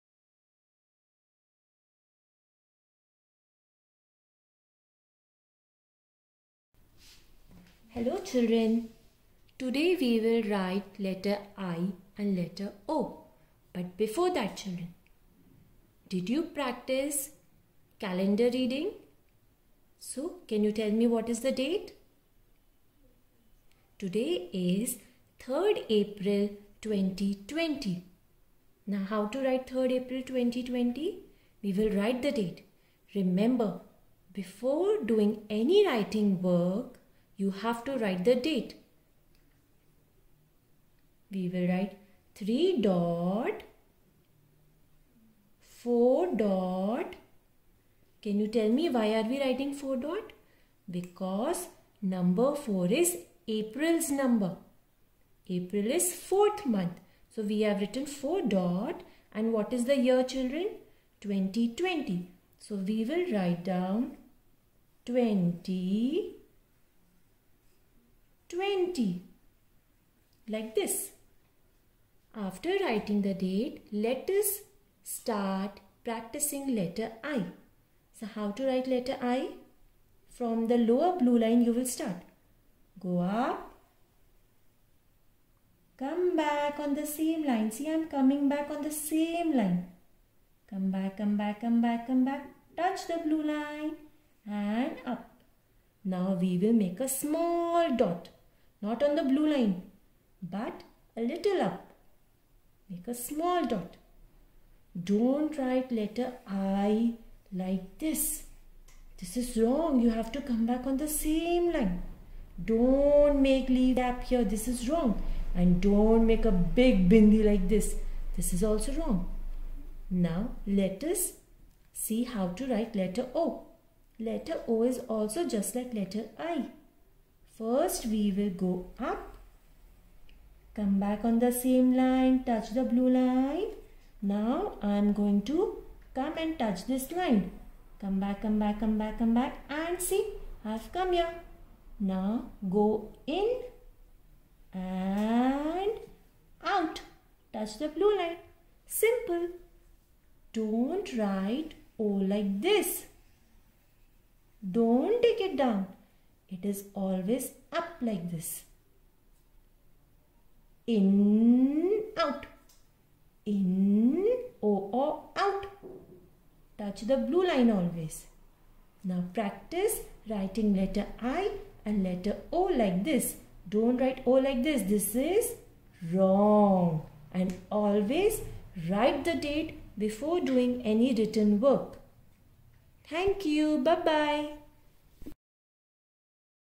Writing letters ' i ' and ' o ' with Charu Ma'am
Practice writing small cursive letters ' i ' and ' o' as shown in the video.